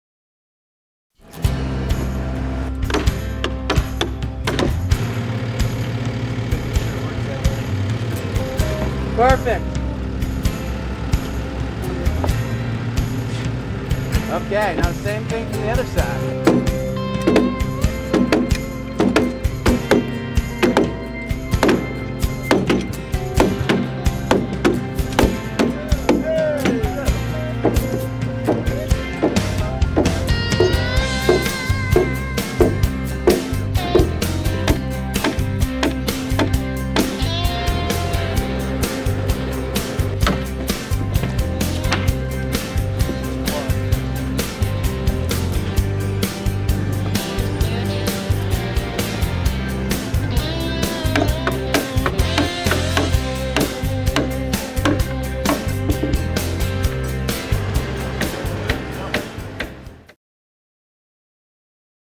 Timber Frame Assembly for a Basketball Barn
Every timber frame barn starts with the assembly. One by one, each timber finds its place in the frame, and oak pegs complete the connections. After assembling all five queen post bents on the ground, the crane will arrive and the raising will begin.

Timber Framing & Commercial Projects
Ellington, CT